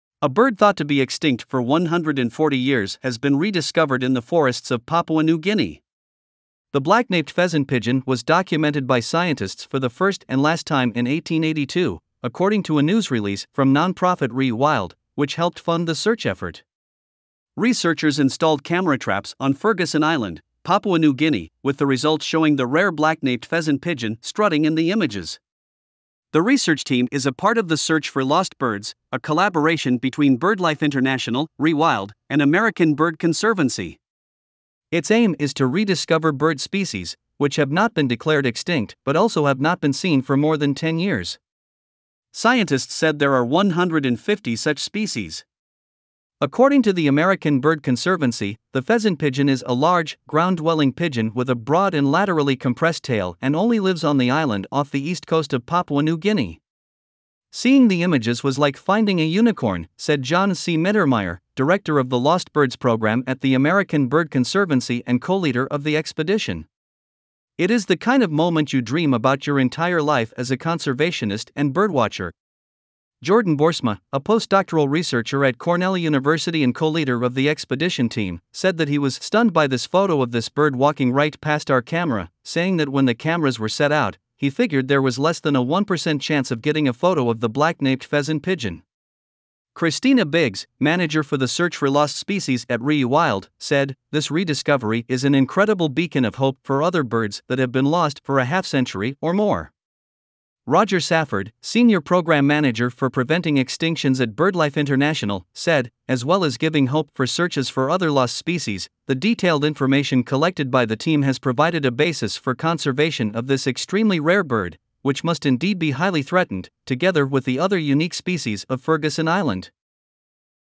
Black-naped pheasant-pigeon sighted in PNG for first time in 140 years
Credits:
Scientists Just Spotted This Large, Colorful Pigeon That No One Has Seen in 140 Years